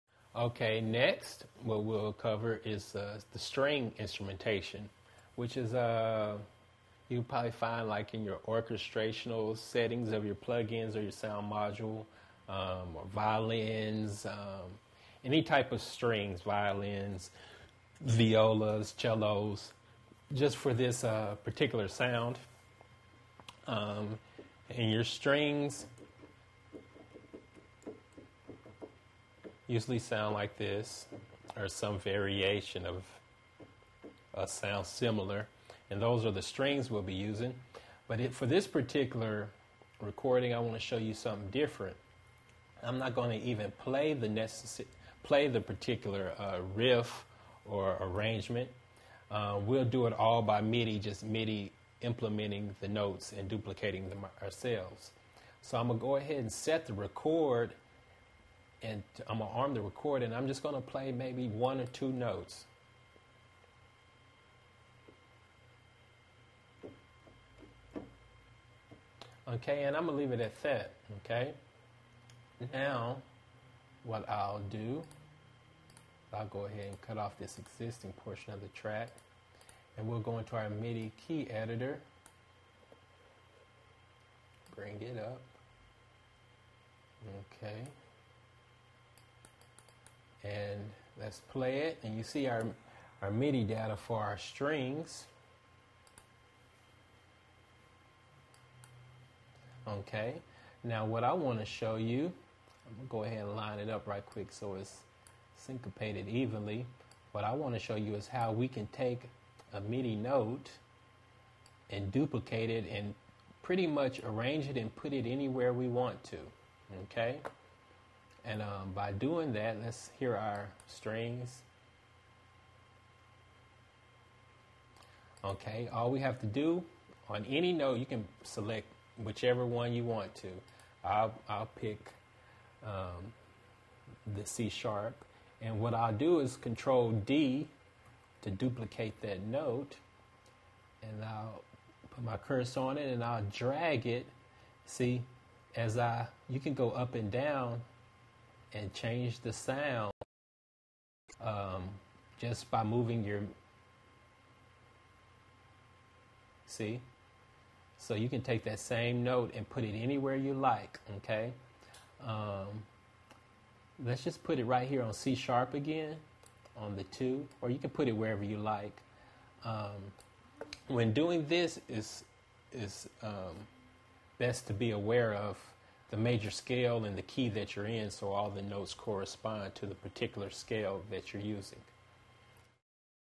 Cubase Tutorial: Editing & Duplicating MIDI Notes
Cubase Tutorial: Editing & Duplicating MIDI Notes. Part of the series: Cubase Tutorial: MIDI Sequencing. Add string tracks to a Cubase MIDI song without playing any instrument, instead using MIDI notes. Learn to duplicate notes for MIDI sequencing in the Cubase DAW from an audio engineer in this free Cubase tutorial video.